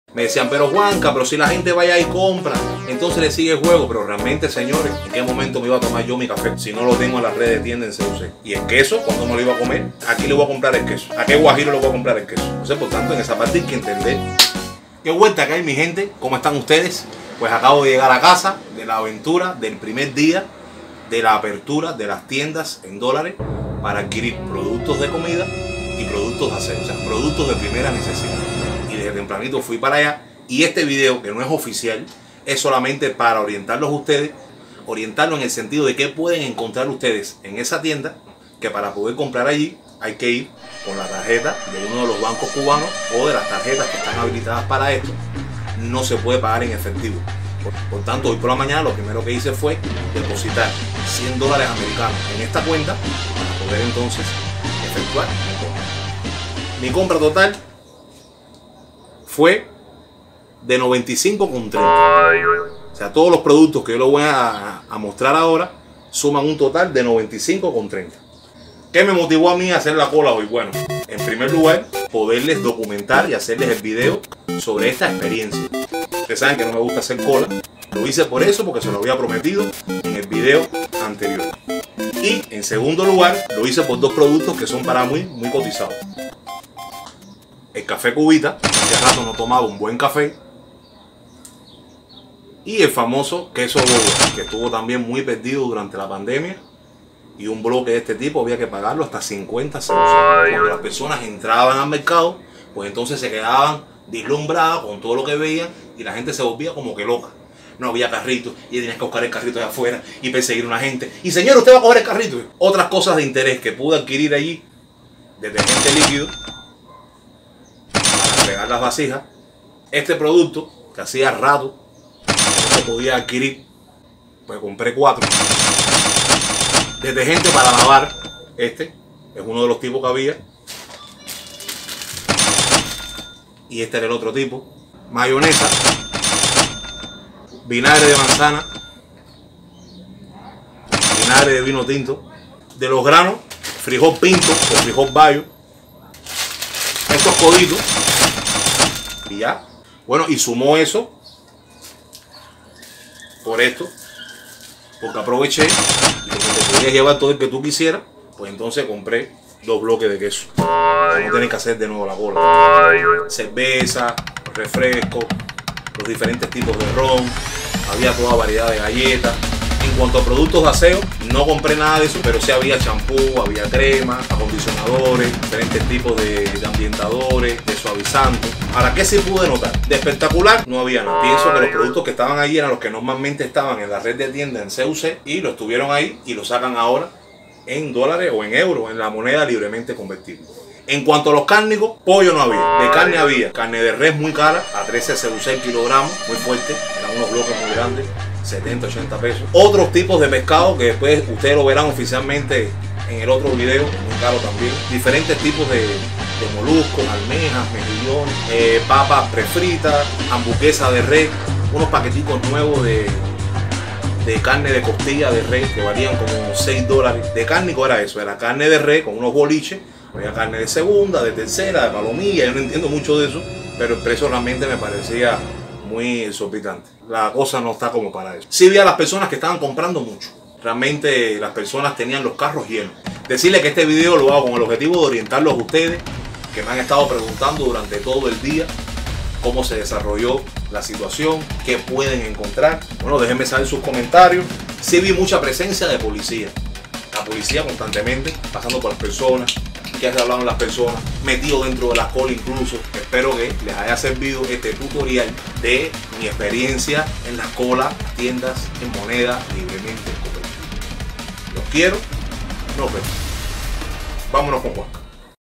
LOS PRECIOS de las NUEVAS TIENDAS EN DÓLARES "Medida Temporal": Han llegado para quedarse, verás...
Gran indignación y espectativa ha causado esta medida del gobierno cubano de vender en Moneda Libremente Convertible productos de mediana y alta necesidad.
Indignación porque no todos pueden acceder a ella y espectativa porque después de meses de escacez total, aparecen productos indispensables que en algún momento pensamos no volver a tenerlos por buen tiempo.
Y de momento aparecieron: pero en dólares!